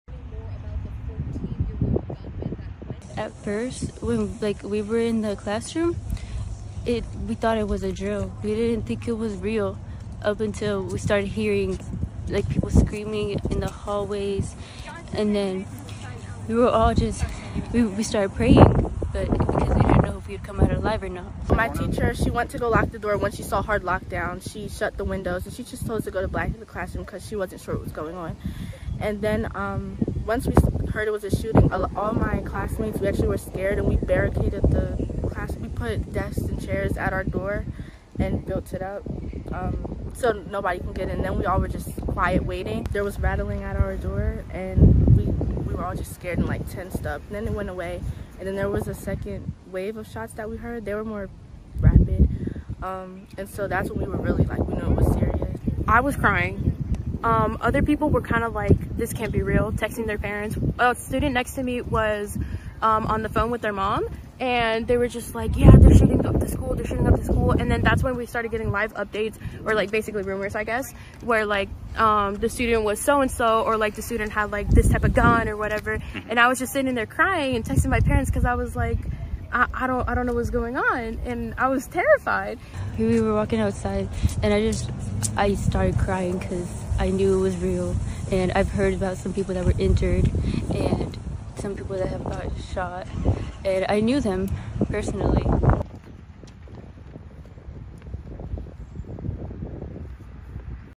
Students describe barricading classrooms during Georgia school shooting
ASSOCIATED PRESS
Winder, Georgia – 5 September 2024
1. Person sitting in front of school sign

ASSOCIATED PRESS
Winder, Georgia – 4 September 2024
2. SOUNDBITE (English) Michelle Moncada, freshman at Apalachee High School:
“At first when we were in the classroom. It we thought it was a drill. We didn't think it was real. Up until we started hearing, like, people screaming in the hallways. And then we were all just we started praying but, because you didn’t know if you’d come out alive or not.”

ASSOCIATED PRESS
3. SOUNDBITE (English) Layla Ferrell, junior at Apalachee High School:
“My teacher. She went to go lock the door when she saw hard lockdown, she shut the windows. She just told us to go to back in the classroom because she wasn't sure what was going on. And then, once we heard it was a shooting, all my classmates, we actually were scared. And we barricaded the classroom. We put desks and chairs at our door and built it up, so nobody can get in and then we all were just quiet, waiting.”

ASSOCIATED PRESS
4. Wide view of people kneeling next to flagpole

ASSOCIATED PRESS
5. SOUNDBITE (English) Layla Ferrell, junior at Apalachee High School:
“There was rattling at our door and we we were all just scared and, like, tensed up. And then it went away. And then there was a second wave of shots that we heard. They were more rapid. And so that's when we really were like, we knew it was serious.”

ASSOCIATED PRESS
6. SOUNDBITE (English) Julie Sandoval, junior at Apalachee High School:
“I was crying. Other people were kind of like, this can't be real. Texting their parents. A student next to me was, on the phone with their mom, and they were just like, yeah, they’re shooting up the school, they’re shooting up the school. And then that's when we started getting live updates or, like, basically rumors, I guess, where, like, the student was so-and-so or like, the student had, like, this type of gun or whatever. And I was just sitting there crying and texting my parents because I was like, I don't I don't know what's going on. And I was terrified.”

7. SOUNDBITE (English) Michelle Moncada, freshman at Apalachee High School:
“We were walking outside and I just I started crying because I knew it was real. And I've heard about some people that were injured and some people that have got shot, and I knew them personally.”

ASSOCIATED PRESS
8. Group of people kneeling next to flagpole
STORYLINE:
Students took shelter as emergency messages flashed across screens to go into lockdown as a 14-year-old student opened fire at a Georgia high school.

“My teacher. She went to go lock the door when she saw hard lockdown, she shut the windows. She just told us to go to back in the classroom because she wasn't sure what was going on,” Layla Ferrel, a junior at Apalachee High School described after the shooting. “We heard it was a shooting, all my classmates, we actually were scared. And we barricaded the classroom. We put desks and chairs at our door and built it up, so nobody can get in and then we all were just quiet, waiting.”

Ferrel said that her classroom door also “rattled” but went away before more “rapid” gunshots could be heard.

Julie Sandoval, another junior at Apalachee High School said she was crying and texting her parents as she hid with fellow students.